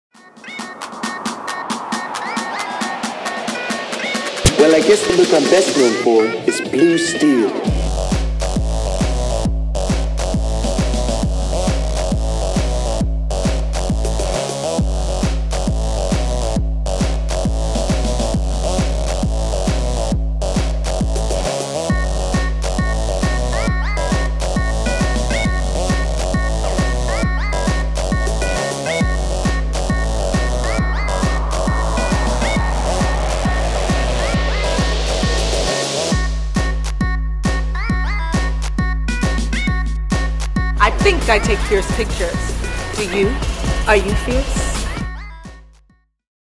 7 Celebs Who Have Signature Red Carpet Poses
You have your favorite pose to rock in pictures and celebs are no different. If you look really closely at all your faves' red carpet pics, you'll notice they have their go-to poses too.